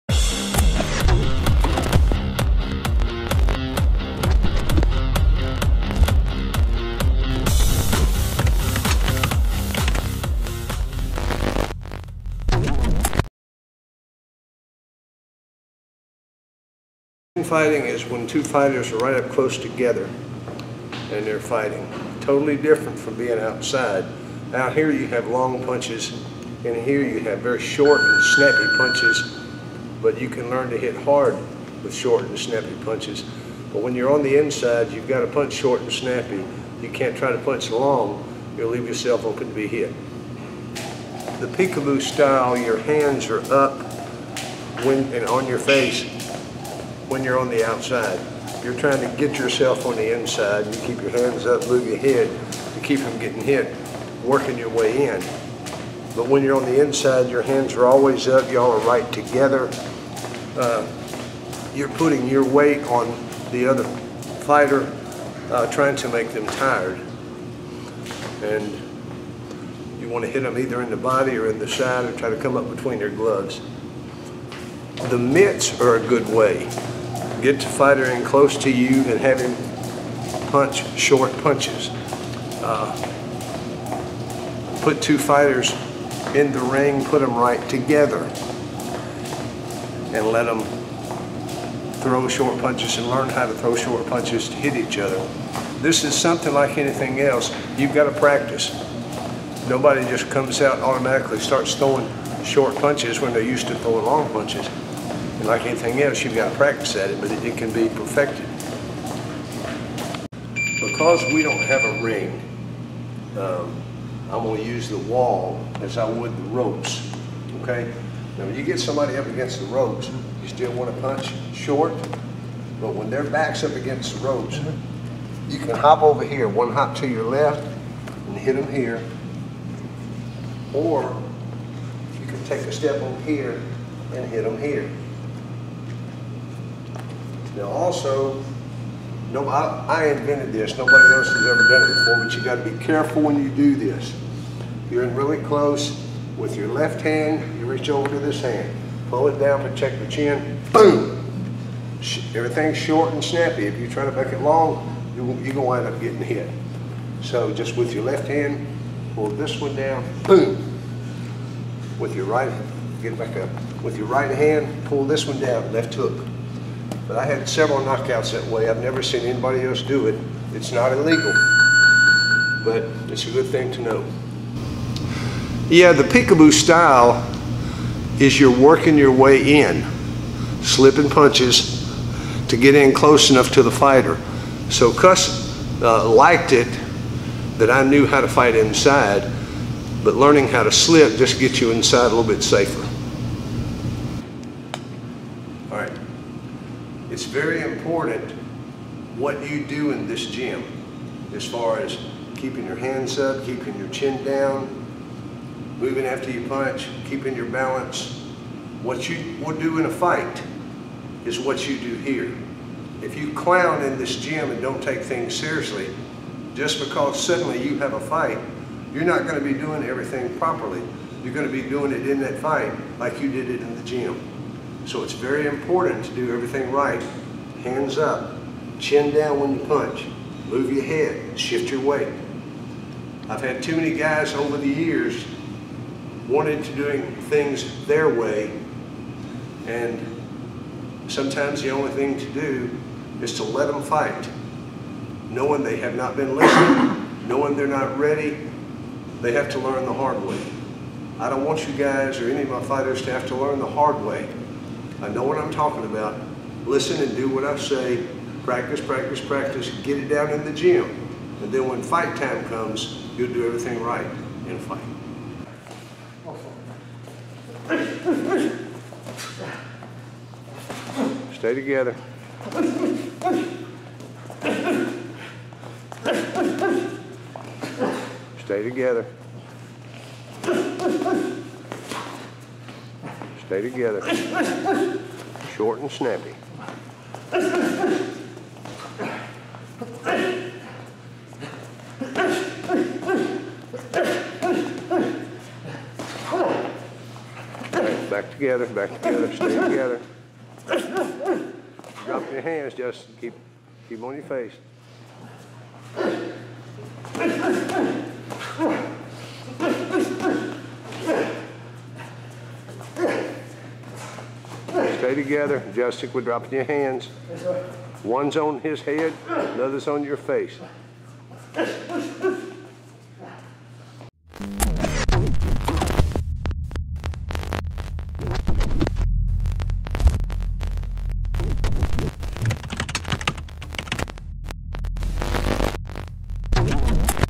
Joey Hadley's AUTHENTIC "PEEK-A-BOO" Boxing Tutorial: Fighting on the Inside
Hadley's Boxing Gym is now closed, but please continue to enjoy these videos and be inspired by the sweet science. Thank you!